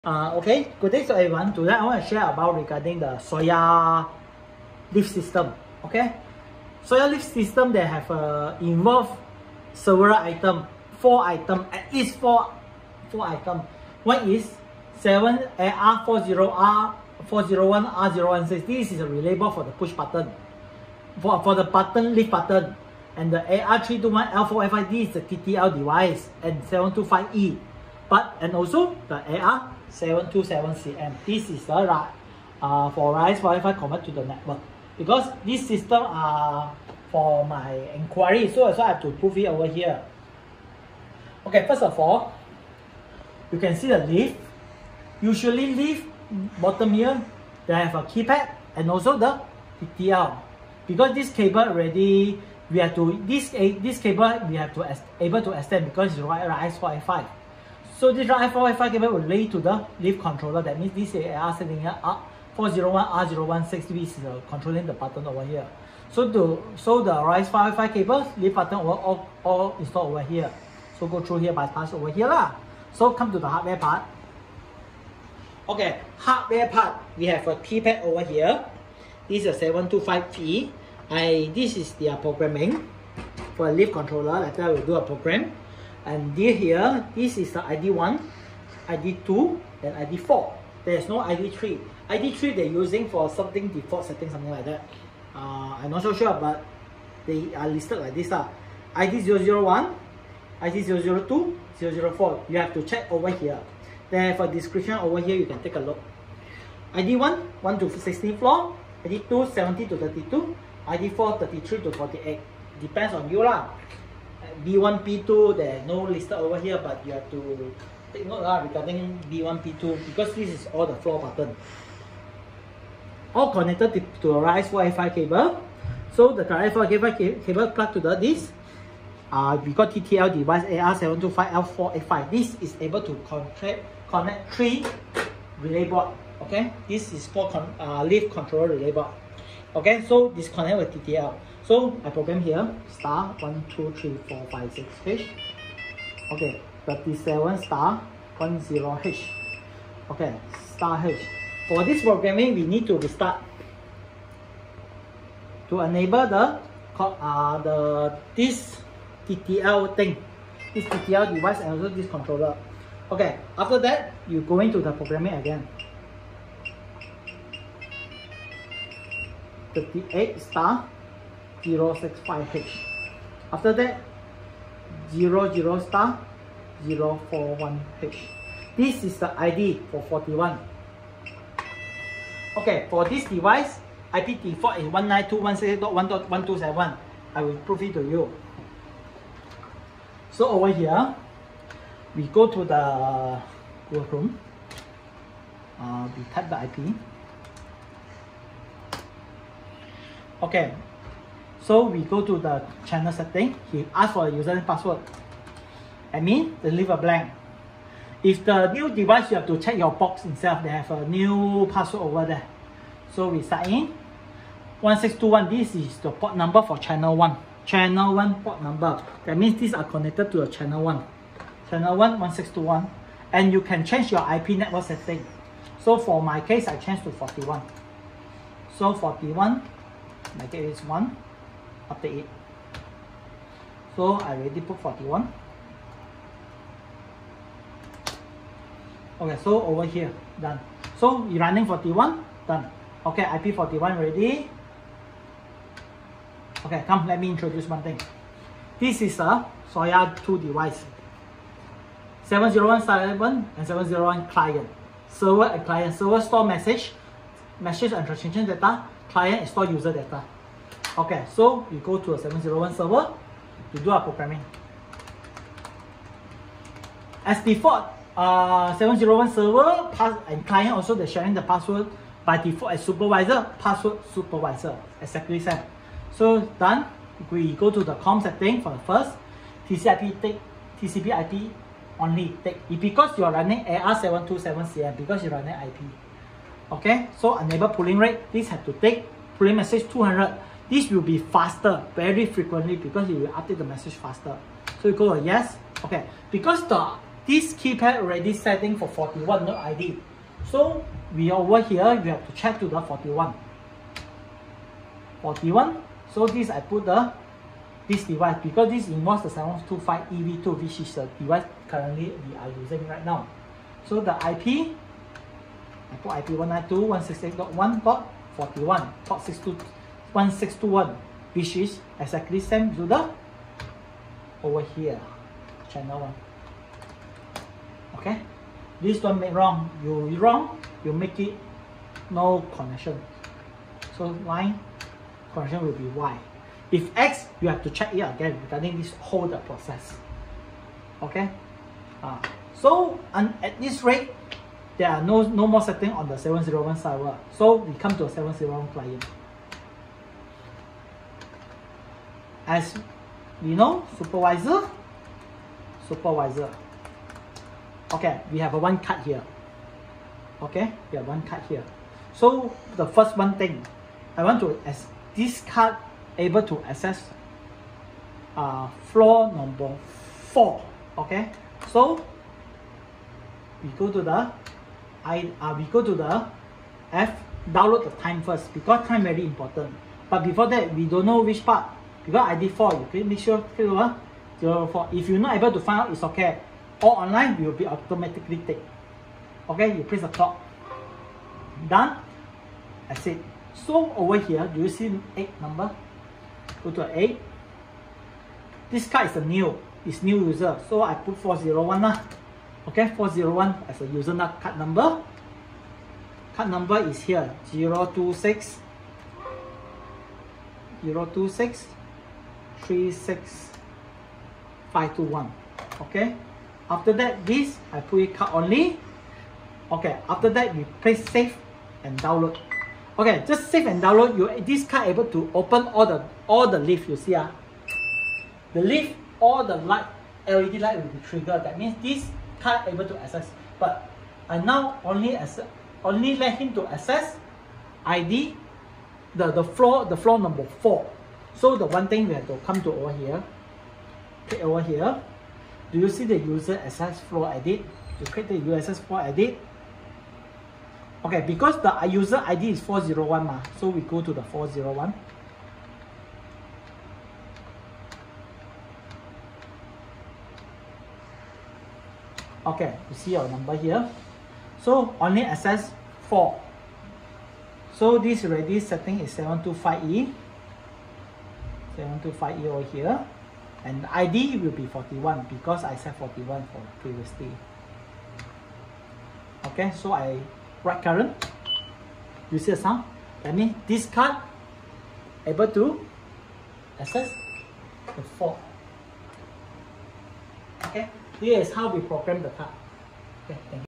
Soyal Lift System: Software 701
The Soyal Lift system (For 1 lift cabin with 48 Floor) are involve with
AR-725E (remember with TTL port) x1
AR-725L485 x1
AR-401R016 x3
AR-727CM x1
PC with software Soyal 701 Server and 701 Client

*remember only AR-725E able connect unit AR-401R016 up 4 unit.
**the AR-721hv3 able connect maximum 2 unit AR-401R016.